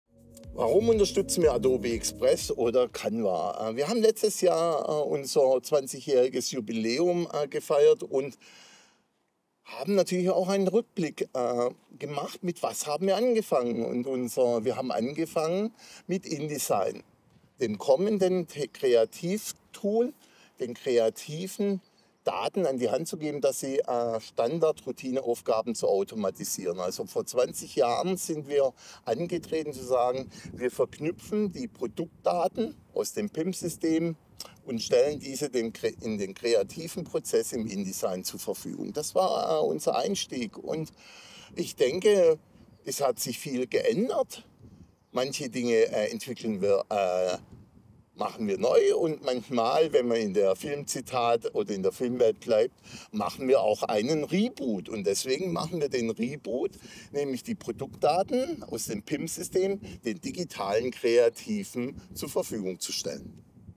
Das neue Adobe Express Add-on der priint Group
Seit 20 Jahren gibt es die WERK II Medien- und Informationsges. mbH | priint Group und angefangen hat alles mit der Unterstützung der Kreativen durch Tools, wie das priint:comet InDesign Plug-in. Heutzutage umfasst das Produktportfolio auch Lösungen für z.B. Adobe Illustrator und so ist es eine logische Konsequenz mit der Zeit zu gehen. Horst Huber, CEO der priint Group, gibt einen kurzen Einblick, wieso genau wir Adobe Express und auch zukünftig Canva unterstützen werden.